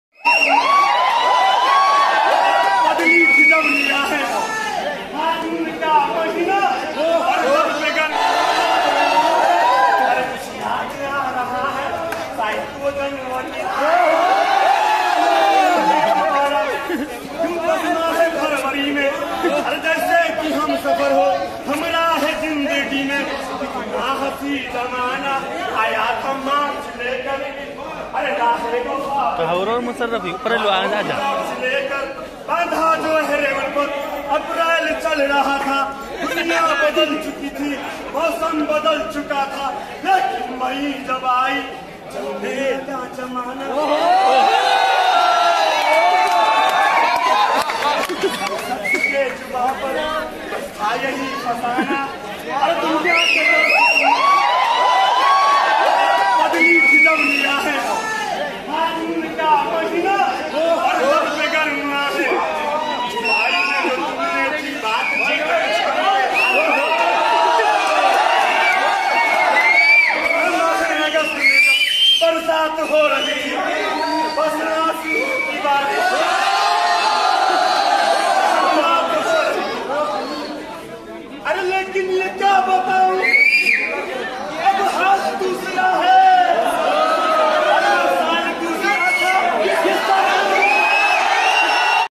Special Program Gaytri Park Talwandi Kota Rajasthan | Allen Kota | Part 2
This Video is of special fun program organised at Gaytri park Talwandi kota rajsthan near Allen Satyarth and Samunnat building .